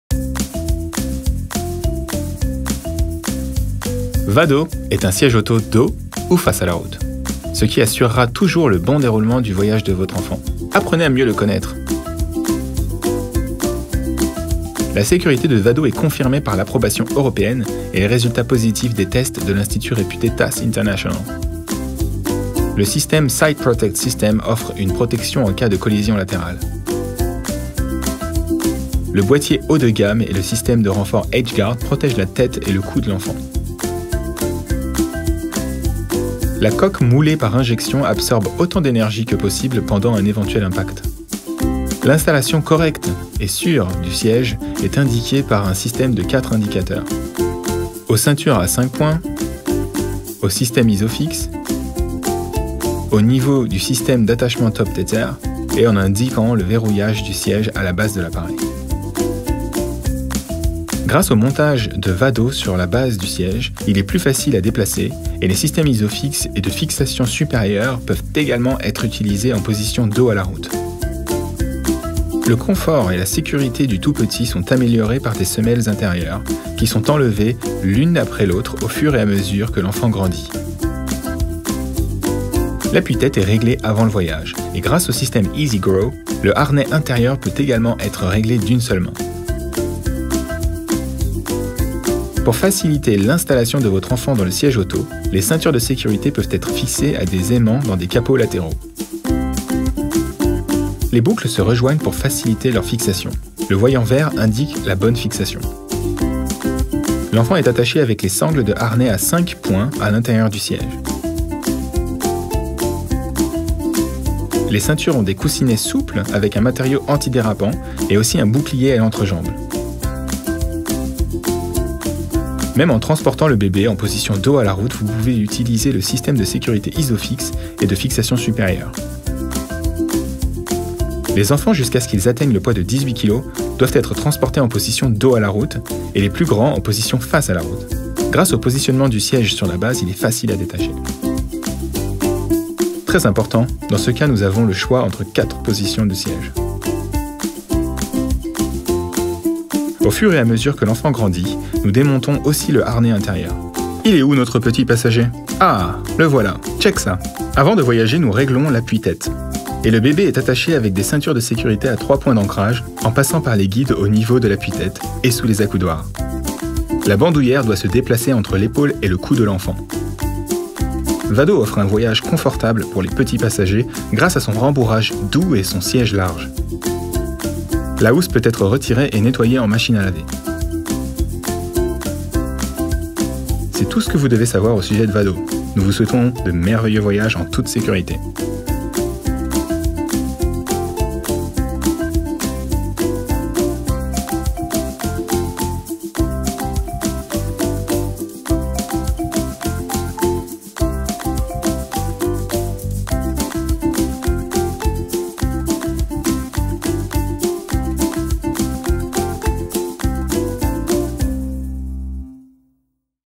Siège auto Kinderkraft Vado
Le siège-auto VADO de Kinderkraft garantit une sécurité maximale pour les plus jeunes voyageurs et la tranquillité d’esprit pour leurs parents. De la naissance jusqu’à 25 kg. A découvrir sur Cabriole